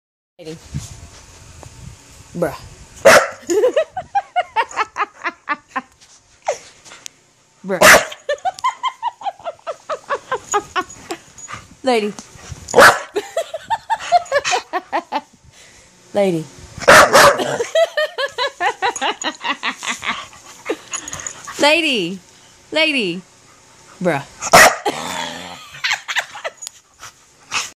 Bruh. Me no is bruh, me is lady!!!
Pitbull doesn't like being called bruh